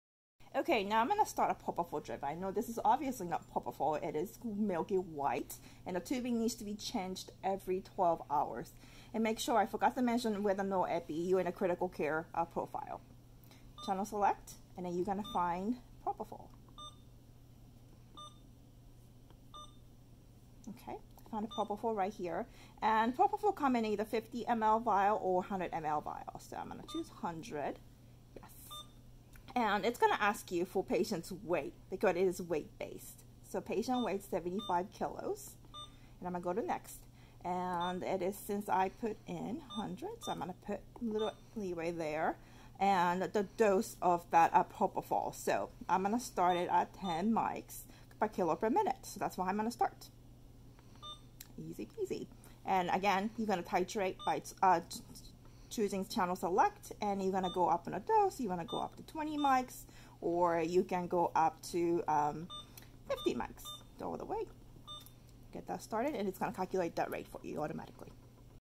Alaris pump programming - propofol drip
I am demonstrating how to program Alaris pump for propofol drip. No actual bottle of propofol was used for this demonstration.